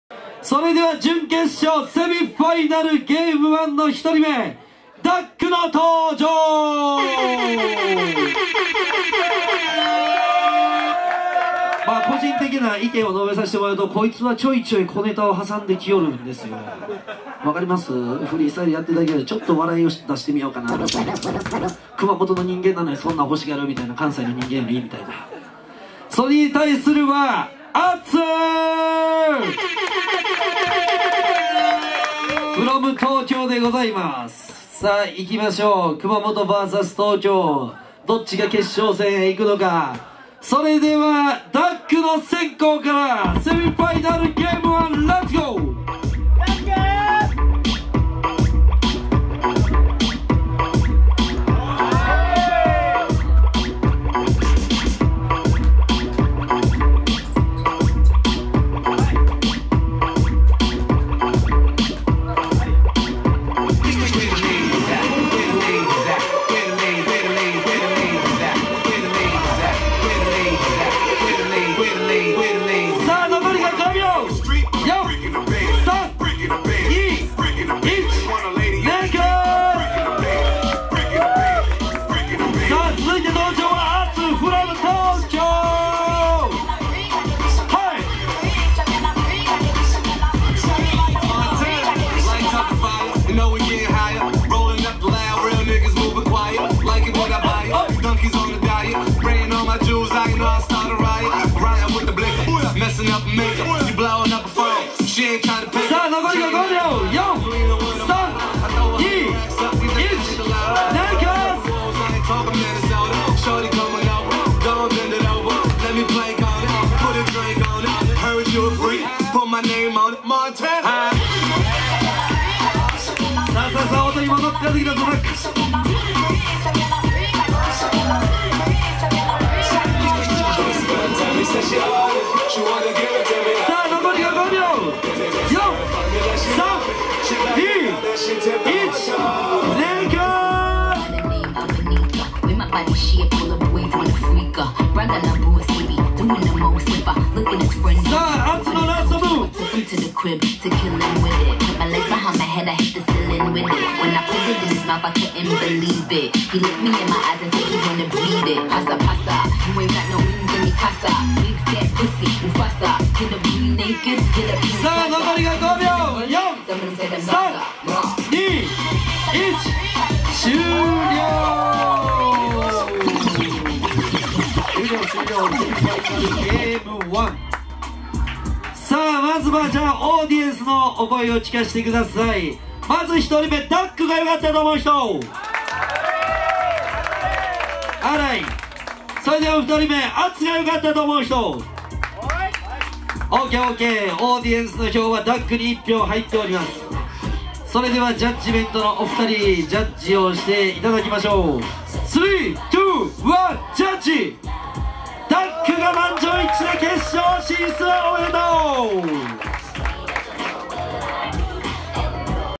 G.r.u #18 SemiFinal1 DAK vs ATSU (Freestyle Basketball Battle)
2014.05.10
G.r.u via BROCKEN @湊町VOLCANO (Osaka, JAPAN)
Freestyle Basketball Battle